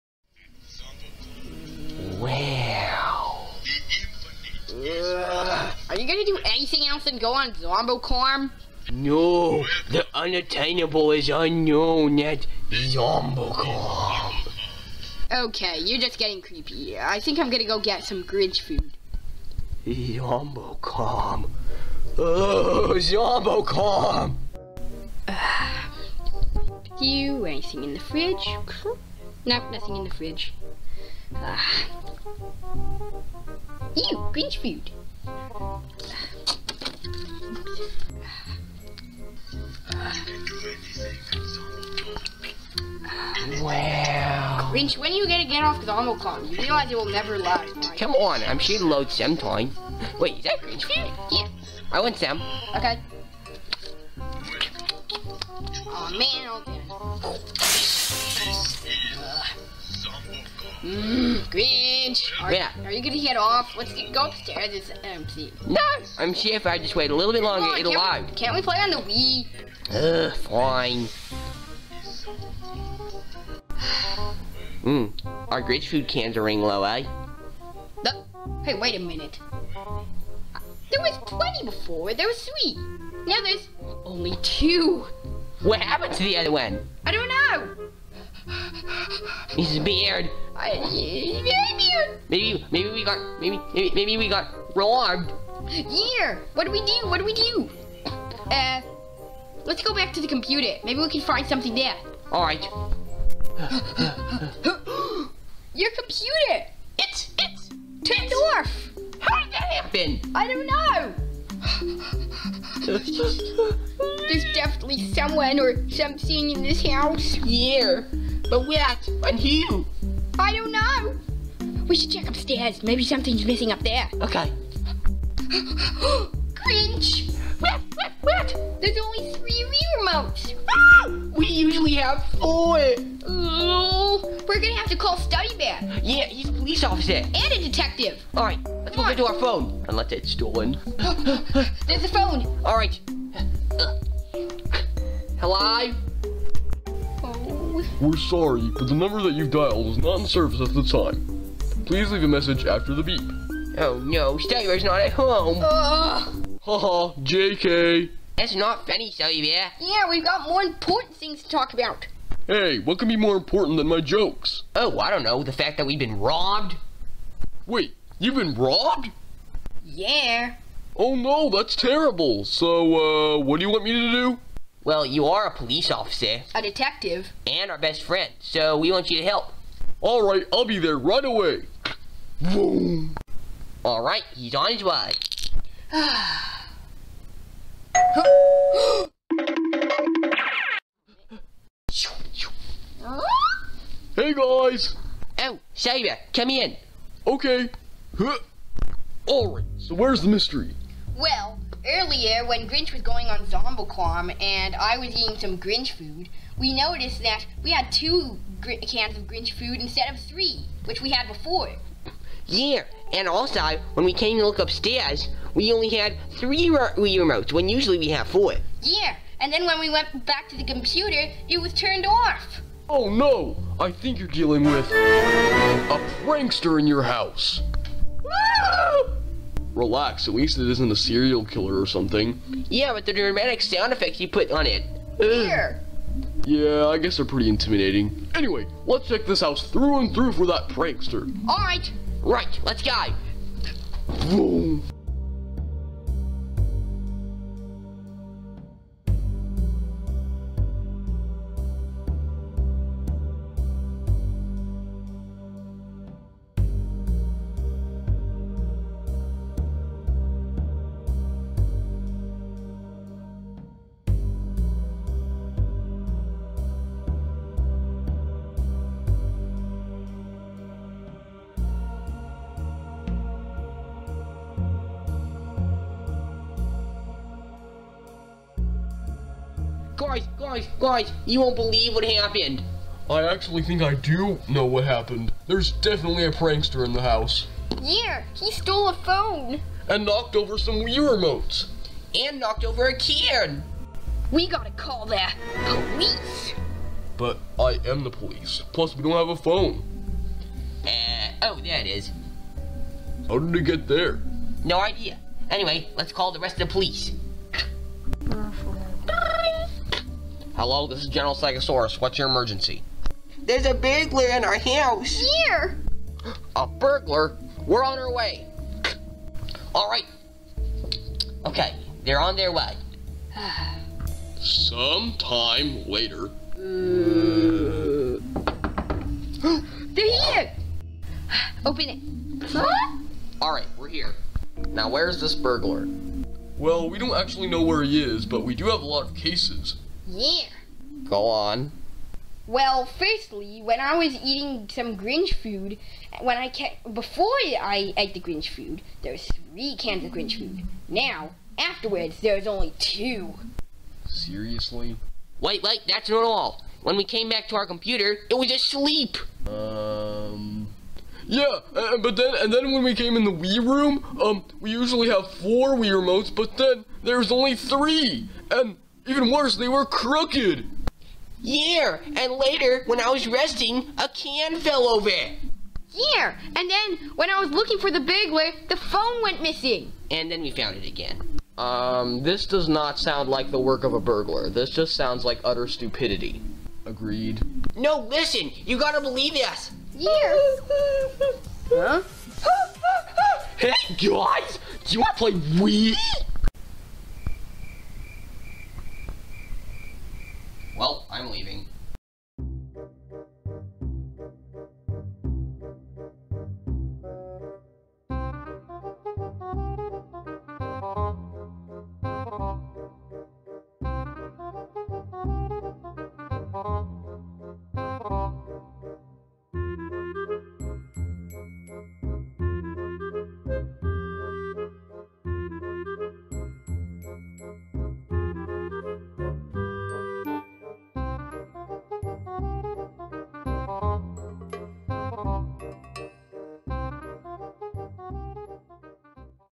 The Burglar
In this video Grinch and Finch start feeling paranoid that their is a burglar in their house, due to things happening inside.